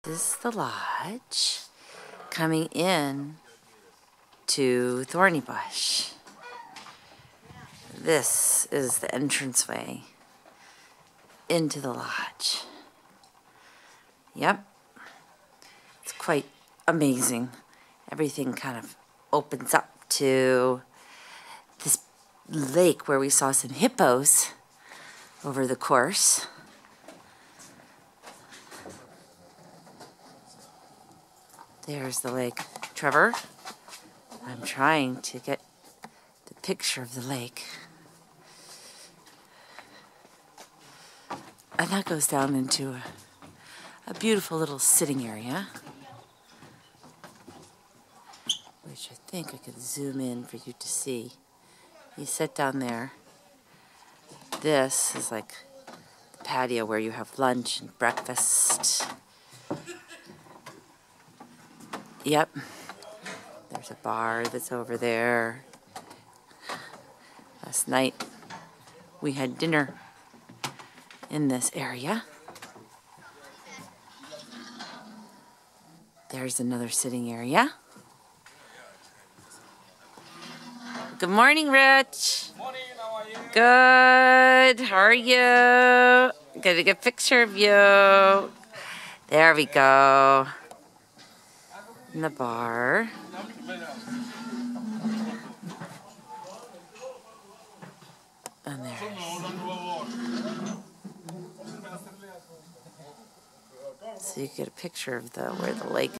South Africa -- Kruger National Park -- Thorny Bush Safari
The pictures of our resort, there was a hippo outside of our room 15 feet away from our door.